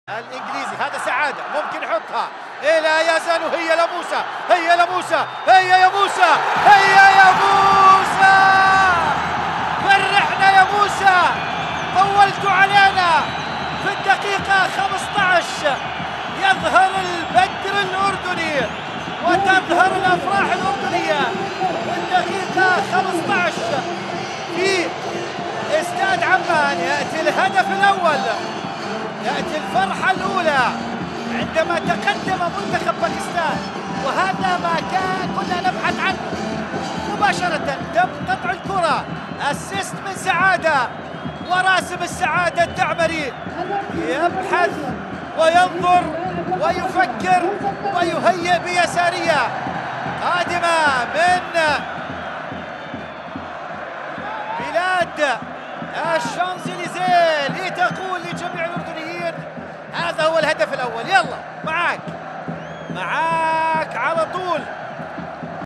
منتخبنا الوطني يحرز الهدف الأول بقدم موسى التعمري في مرمى المنتخب الباكستاني
منتخبنا الوطني يحرز الهدف الأول بقدم موسى التعمري في مرمى المنتخب الباكستاني  ضمن التصفيات الآسيوية المشتركة